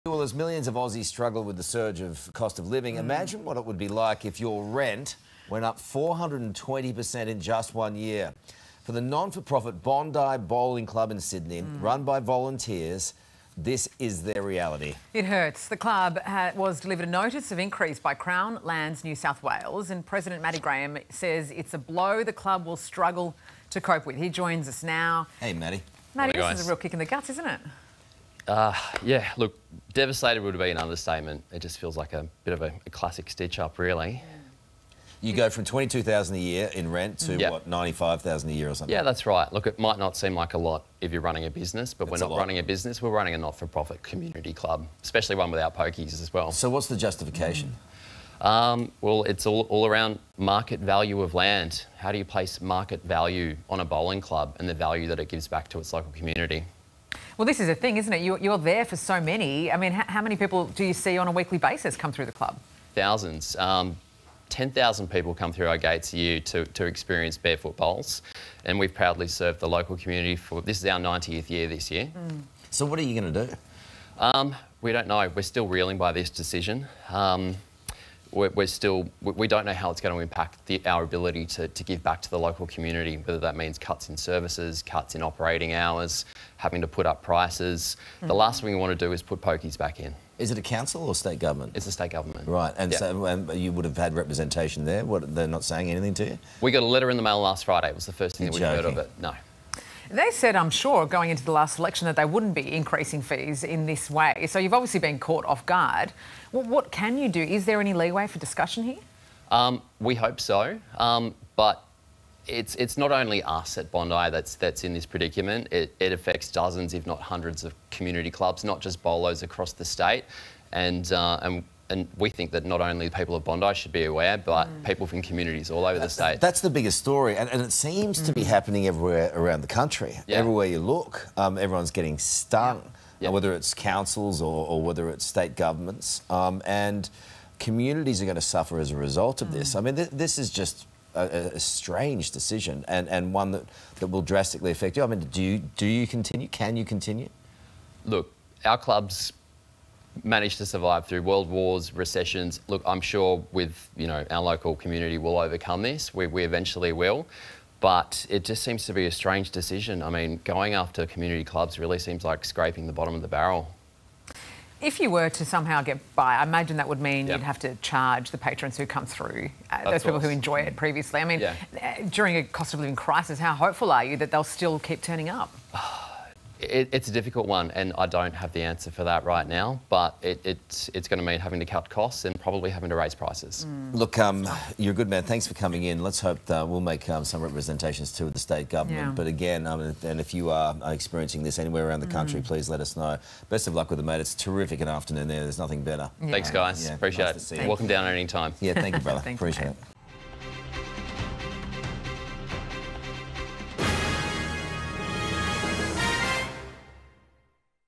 Bowling club hit with 420% rent increase | 9 News Australia
As millions of Aussies struggle with the surging cost-of-living, the non-for-profit Bondi Bowling club has been hit with a 420% rent increase in just one year. The bowling club is run by volunteers and President Matty Graham says the increase is a struggle to cope with.